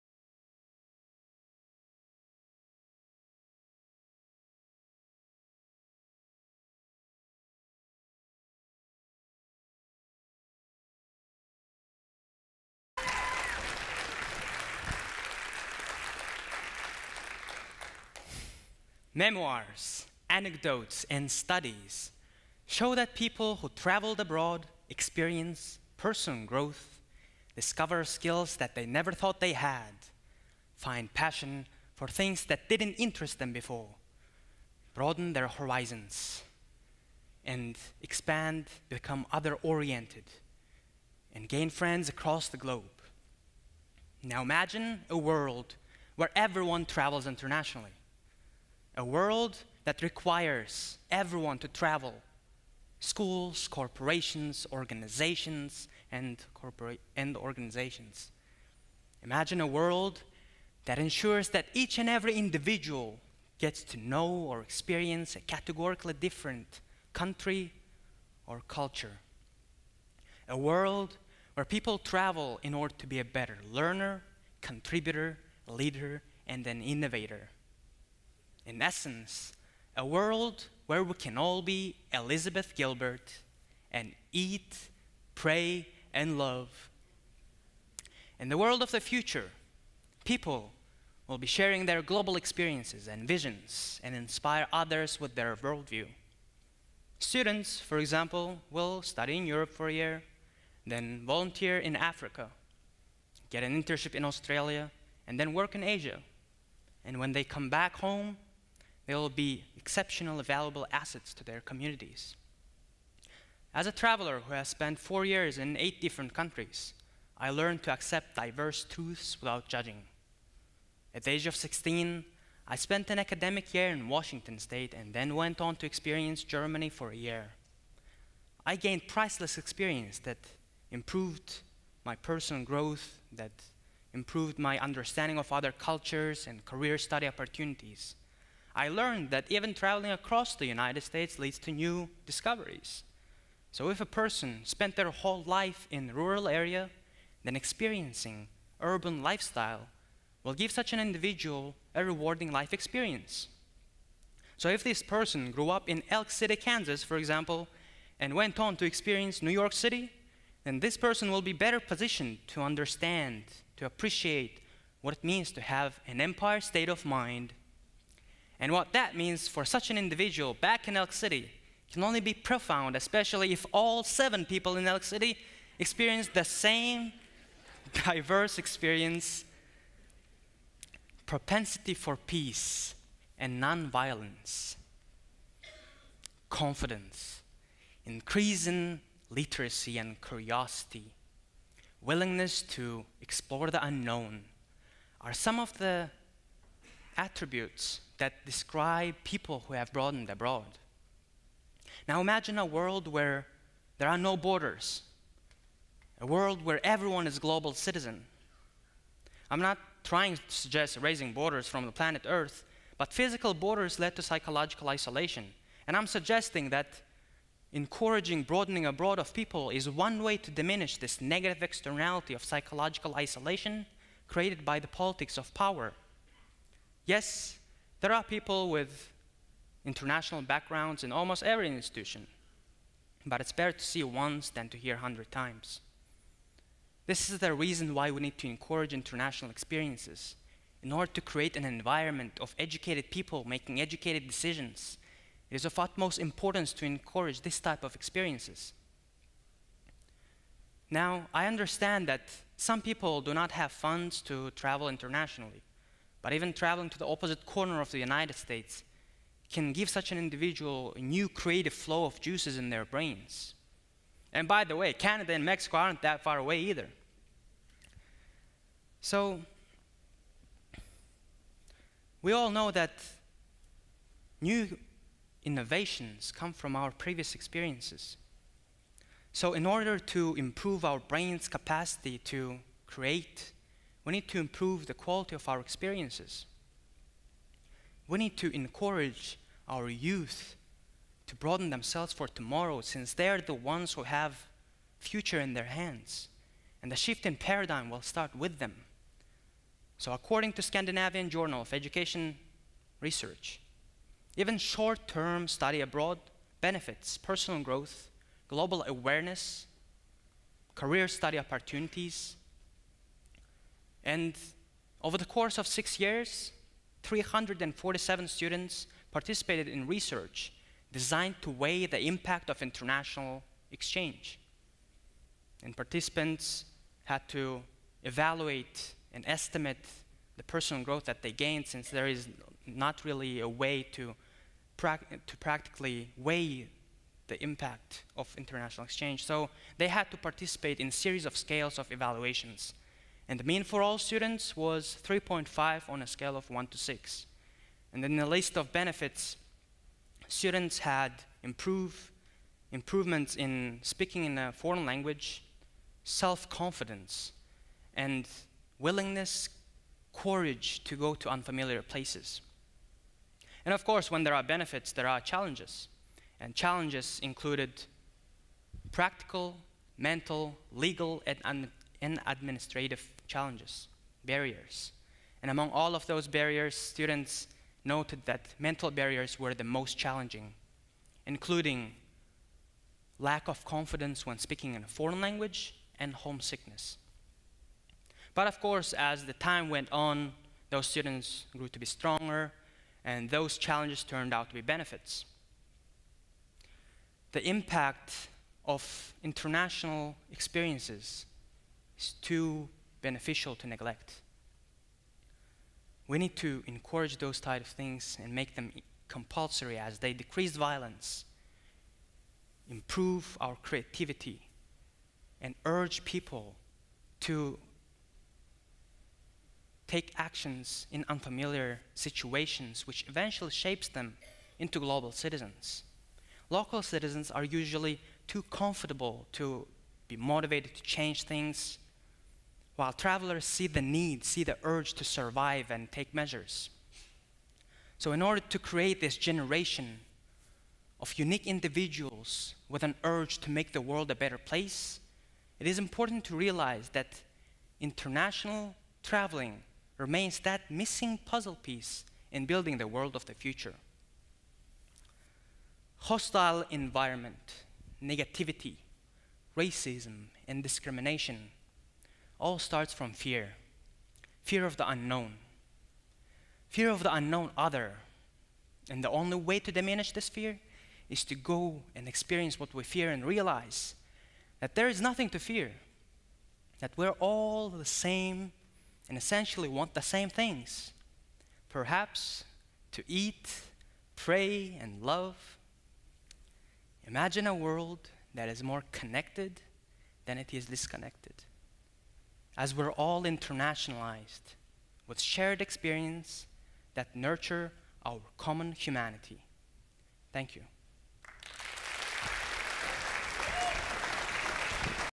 A Broadening Abroad - An International Prerequisite | Nurali Mamedov | TEDxICC
This talk examines the benefits one would experience if required to travel.

Nuraly is a 20-year-old international student from Turkmenistan. He was 16, when he got selected to be a finalist of Future Leaders’ Exchange Program. He spent the following academic year being a representative of Turkmenistan in Washougal High School in Washington State. After a year in the USA, he came back to his home country to finish high school. During his last school year, he became a finalist of the Junior Achievement Asia-Pacific Company of the Year 2013, where he served as the President of a student-run company. As a result of the competition in Kuala-Lumpur, Malaysia, among 17 countries, their company got both the first place and the FedEx Access Award. Mammedov  is student at Independence Community College. He is planning to major in Industrial Engineering and further transfer to a four-year school. He became a Coca-Cola Global Leader of, as well as an All-Kansas Academic Team Scholar. Currently, Vice-President of Debate/Speech, Kansas-Nebraska Regional President of PTK.